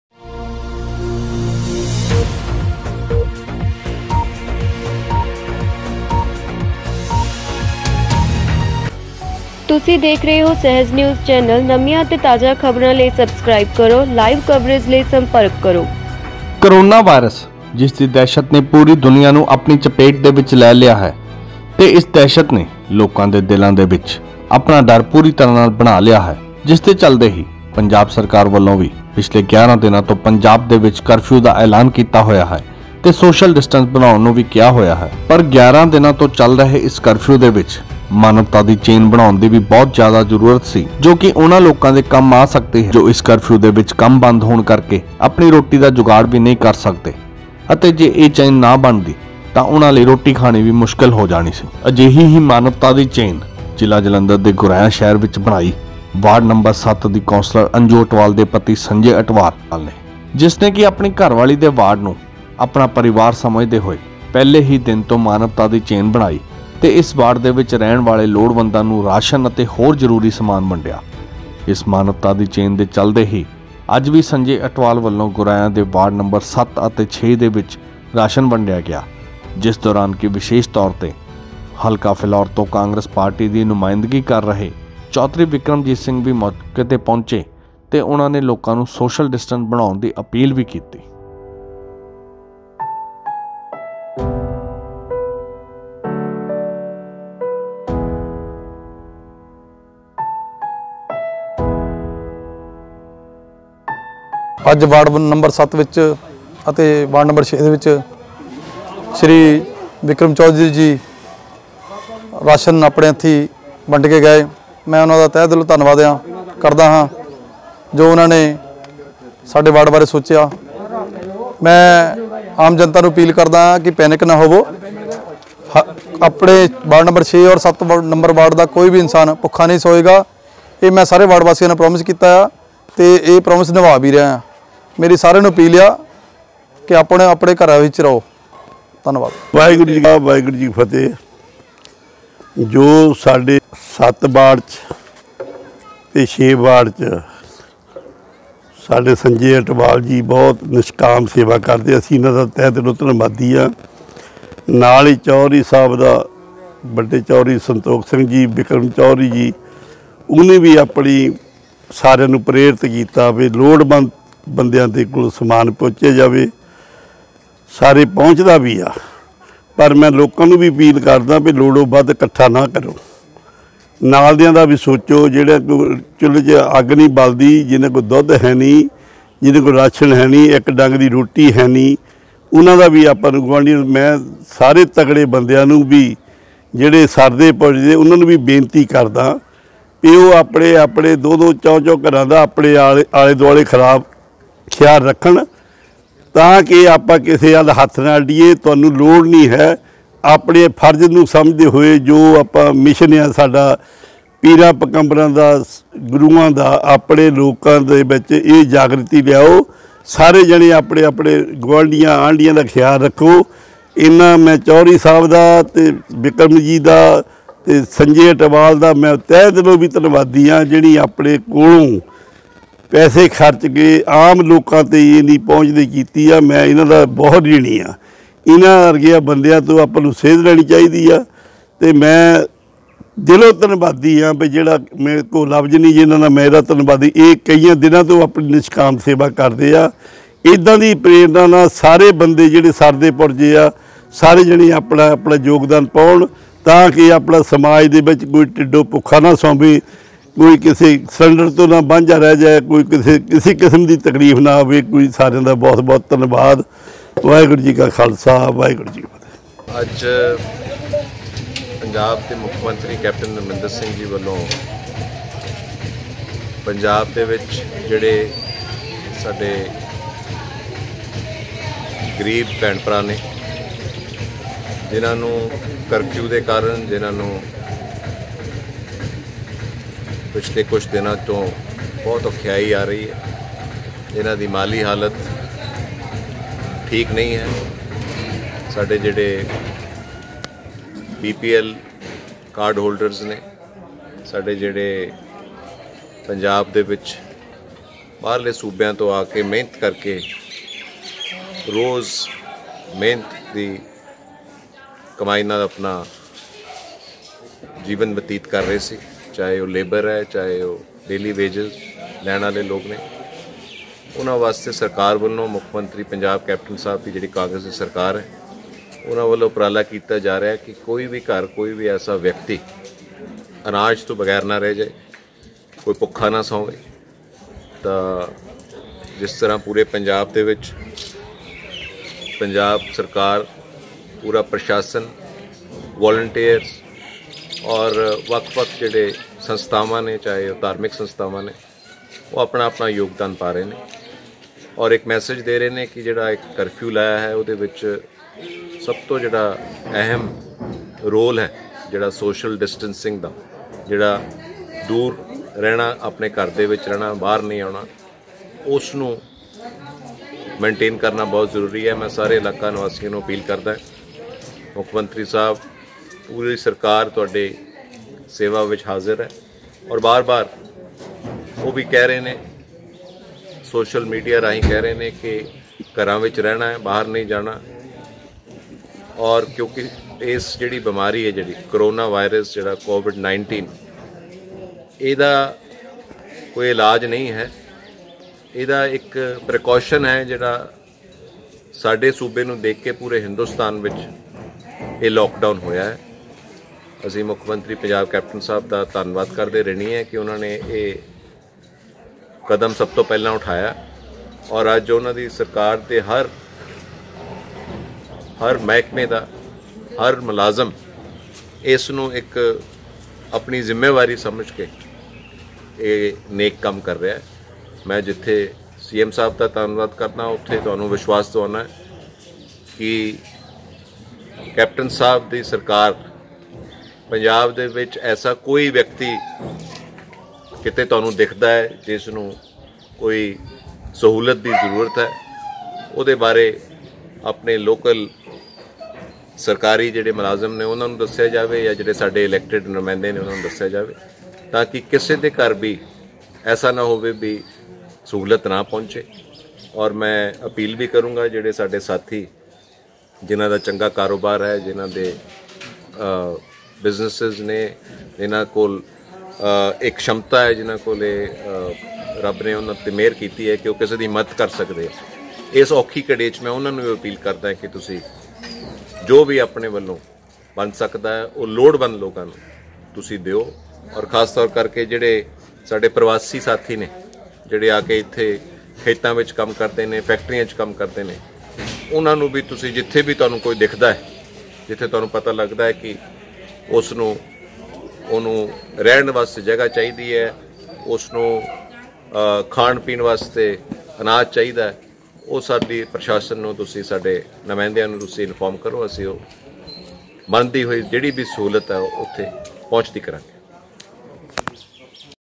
ਕੌਂਸਲਰ ਅੰਜੂ ਅਟਵਾਲ ਅਤੇ ਸੰਜੇ ਅਟਵਾਲ ਵਲੋਂ ਵਾਰਡ ਨੰਬਰ 6 ਅਤੇ 7 ਲੋੜਵੰਦਾਂ ਨੂੰ ਘਰਾਂ ਚ, ਜਾ ਕੇ ਰਾਸ਼ਨ ਵੰਡਿਆ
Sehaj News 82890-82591